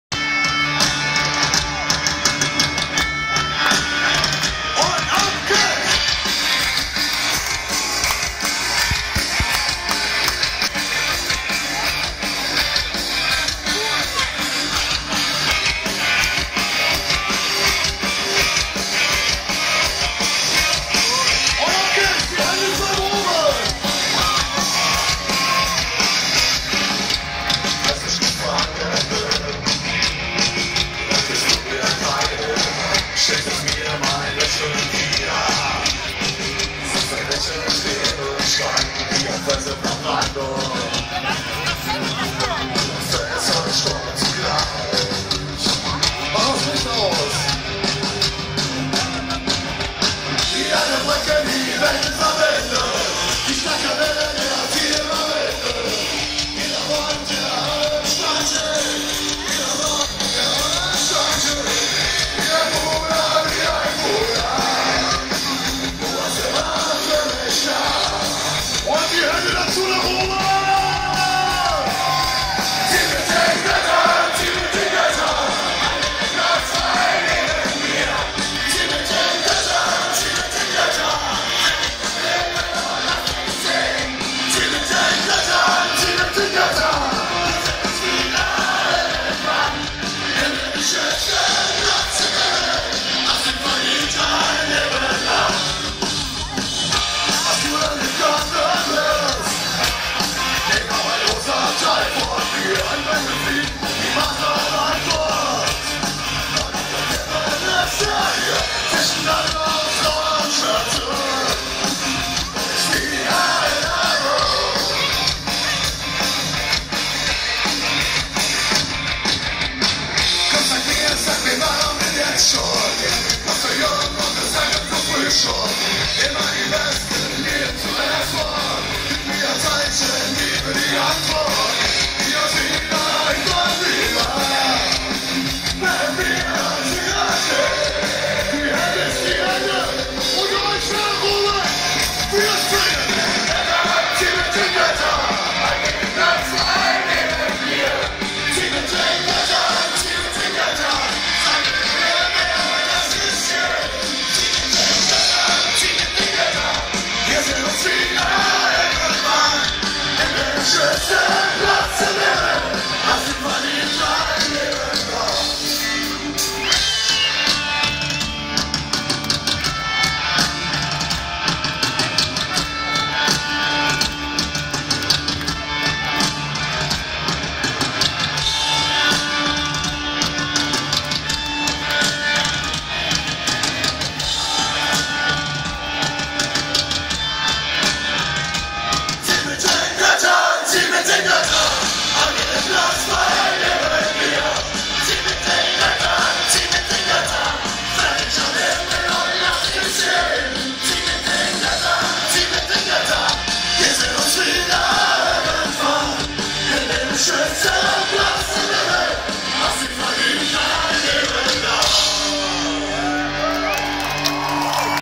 Frei.Wild Wulheide 30.07.16 Handy APP Action
in dem Video wurde die Frei.Wild App Lightshow activirt
und die Hanhys leuchten zum Song in verschiedenen Farben